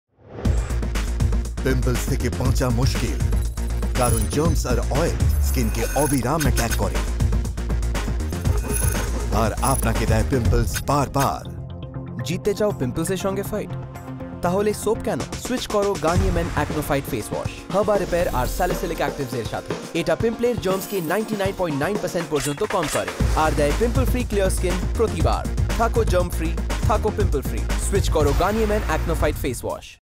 Garnier men Acnofight Facewash
If you are suffering from pimples and have dark spots, please try our Garnier Men Acno Fight 6 in 1 pimple clearing face wash. It helps control oil, dry pimples, as well as uproots blackheads, tighten pores, reduces redness and lighten marks.

 the price of Garnier Men AcnoFight 6in1 Pimple Clearing Face Wash 50g and 100g pack is respectively 110 Taka and 210 Taka only.